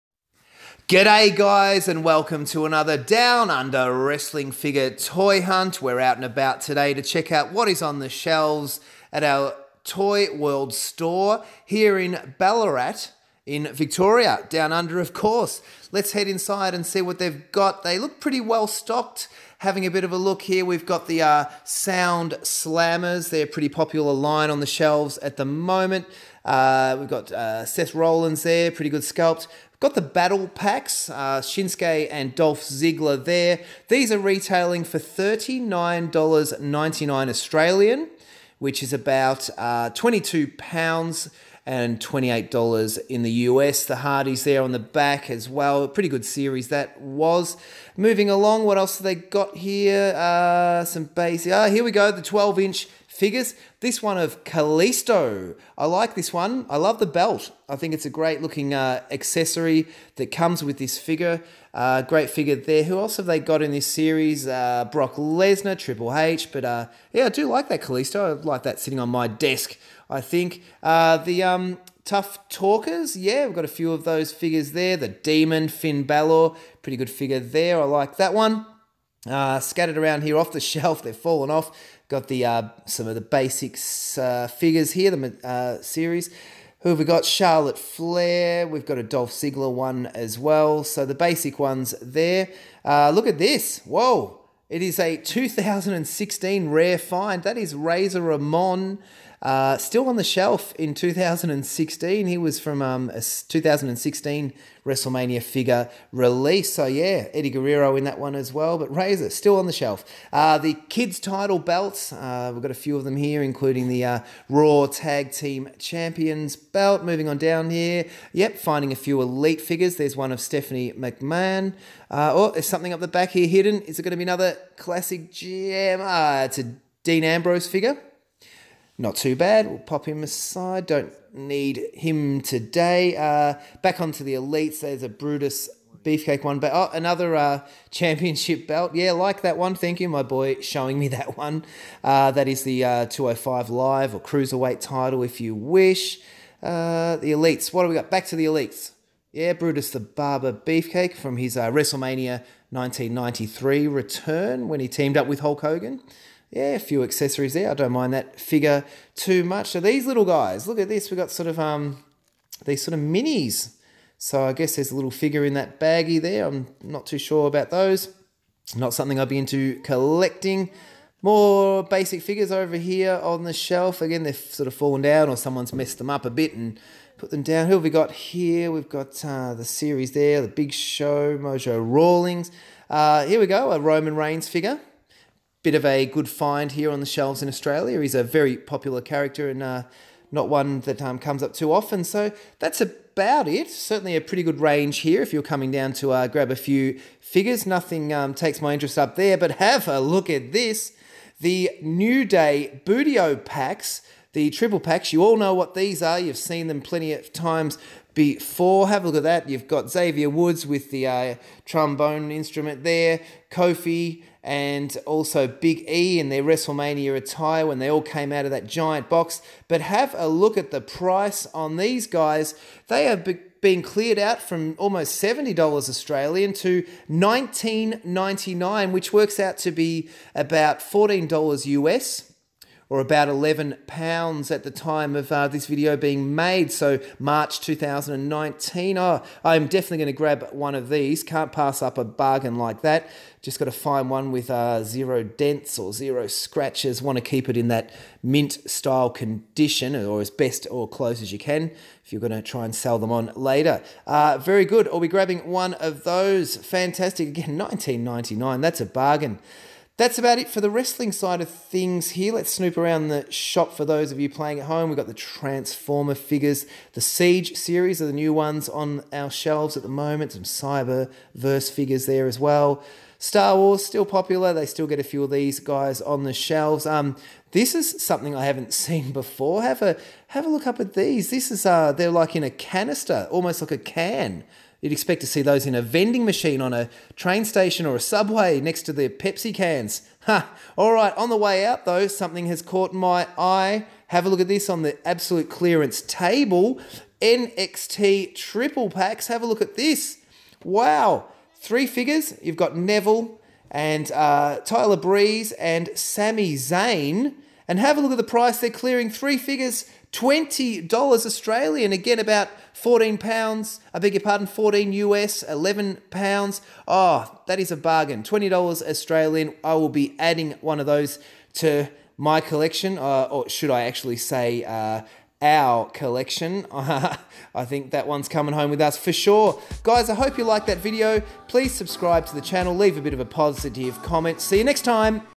TOY HUNT - NEW DAY BOOTY O's 3 Pack WWE Clearance SALE !! Mattel Elite Wrestling Figures DOWN UNDER
What WWE Wrestling Figures do we have DOWN UNDER in Australia ??? Well in this editions Toy Hunt (March 2019) ... we are at Toyworld Ballarat checking out what figure are available. I hope you enjoy this clip I've put together ...